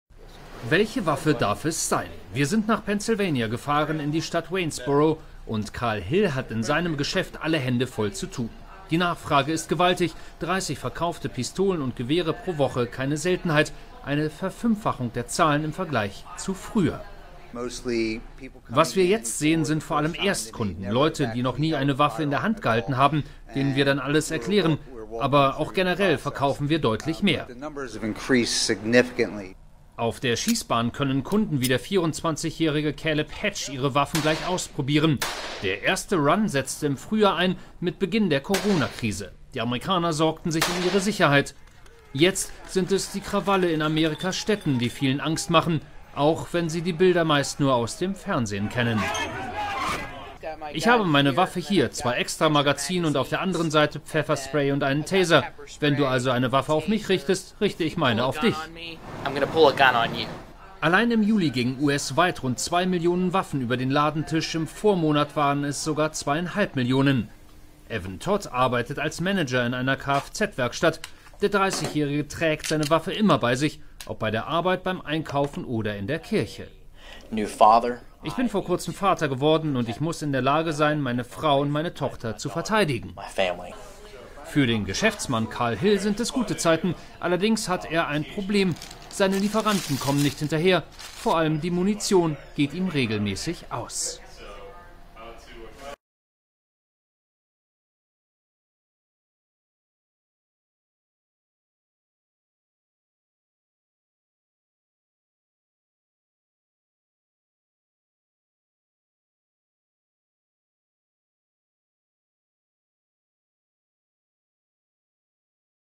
NICHT NUR TRUMP-FANS: Kurz vor den Wahlen kaufen Amerikaner wie verrückt Waffen und Munition
Die Amerikaner kaufen zurzeit Waffen als gäbe es kein morgen mehr. Der US-Wahlkampf, Corona, die BLM-Proteste und eine allgemein zunehmende Aggressivität treibt auch Bürger ohne spezifische Waffenkenntnis in die Gun-Shops. Mittlerweile gibt es schon Lieferengpässe bei besonders beliebten Waffen und Munition, klagen ansonsten hochzufriedenen Waffenhändler. Unser Reporter Steffen Schwarzkopf hat in Pennsylvania Waffen-Käufer nach den Gründen für ihre Kaufentscheidung befragt.